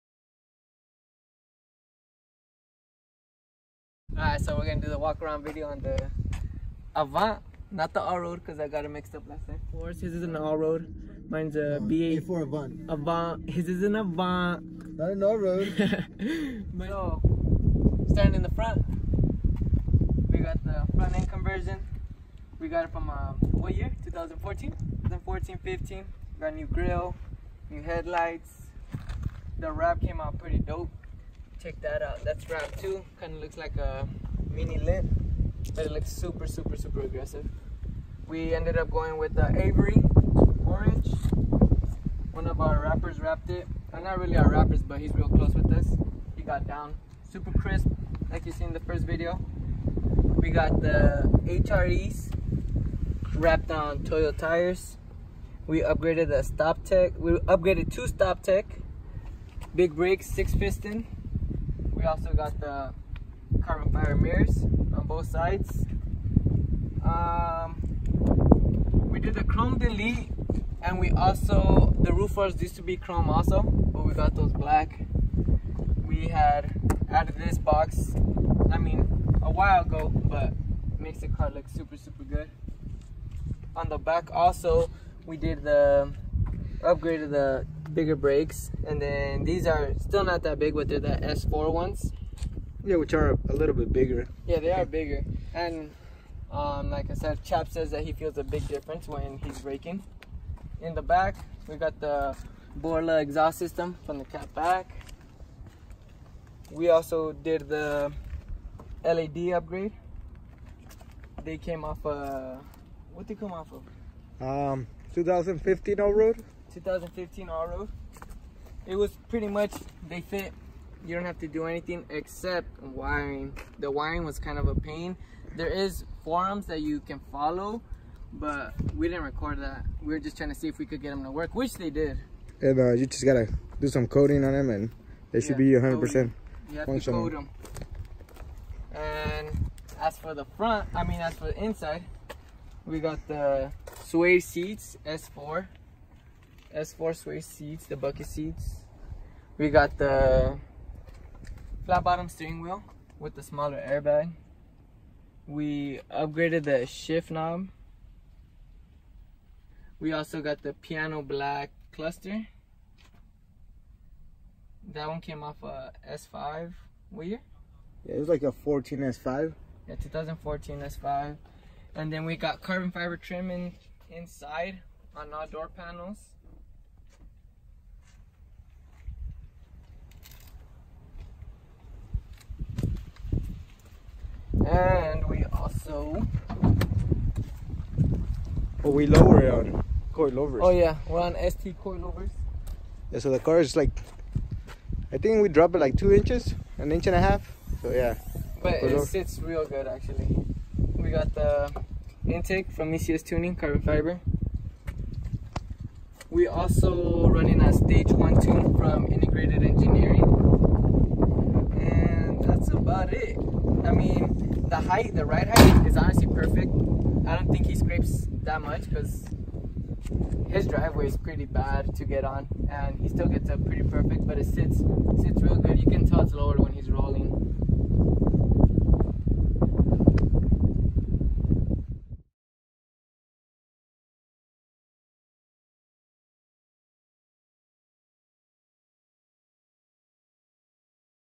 A4 AVANT FACELIFT WALK AROUND AND MODS.
HI EVERYBODY, THIS IS MY 2010 Audi A4 AVANT, WITH FACELIFT AND SOME OF MY MODS,

BORLA
ECS TUNNING
STOP TECH
HRE WHEELS
TOYO